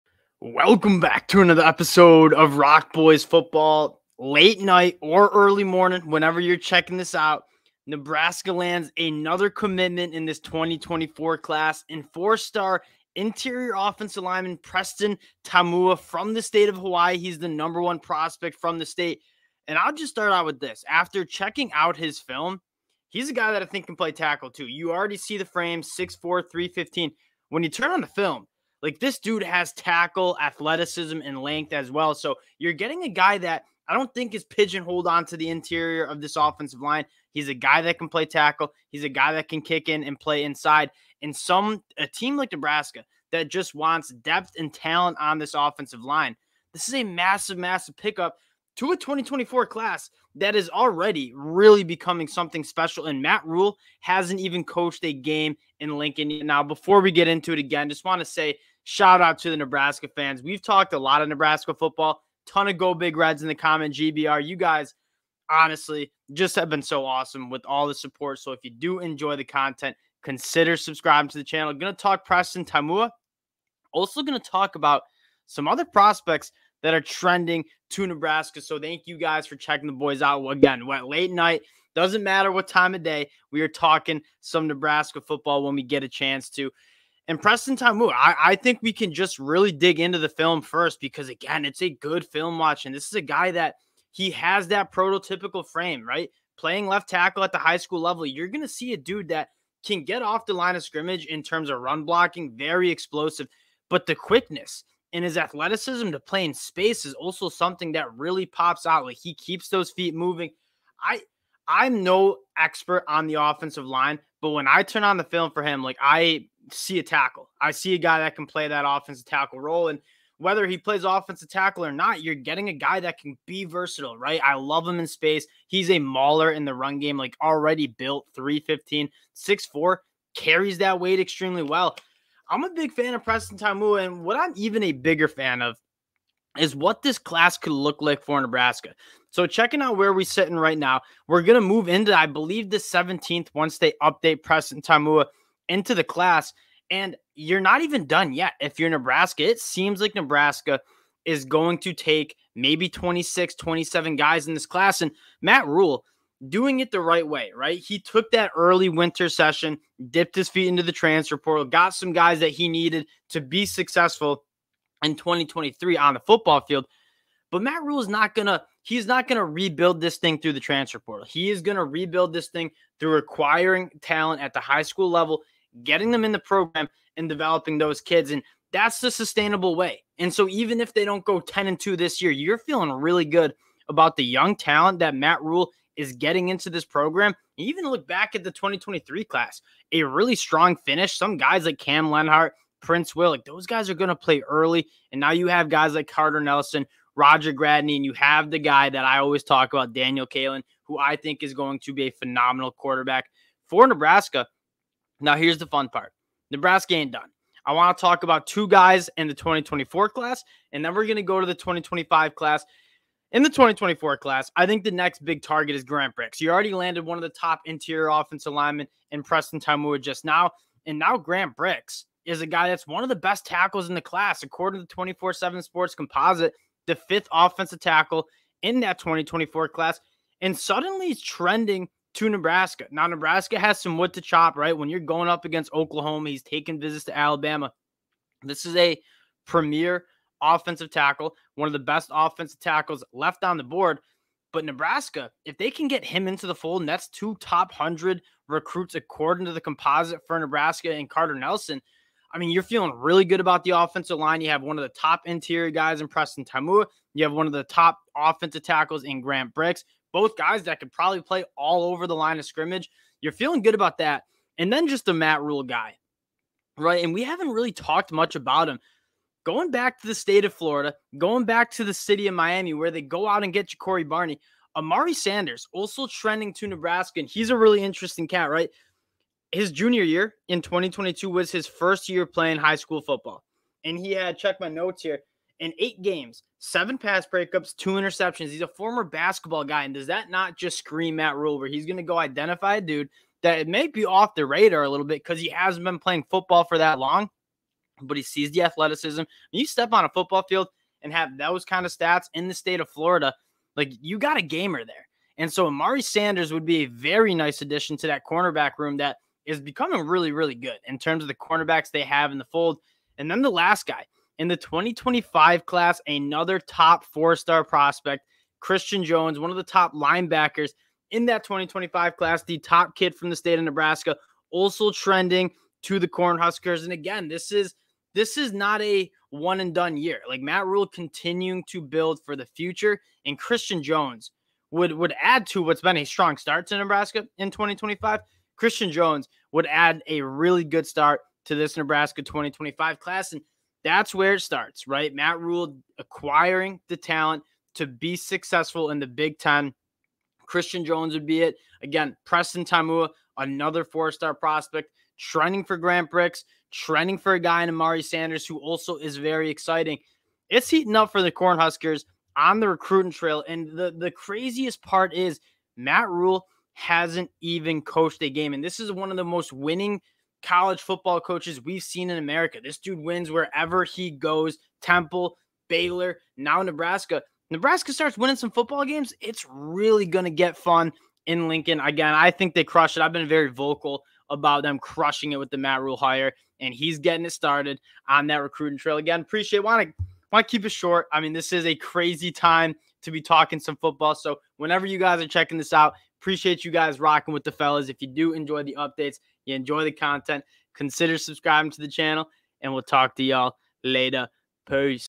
Preston Taumua Commits to Nebraska | Who is Next for Matt Rhule and the Nebraska Cornhuskers?
Matt Rhule and the Nebraska Cornhuskers Football Program land a massive commitment in 4 star OL Preston Taumua from the state of Hawaii. Grant Brix next up?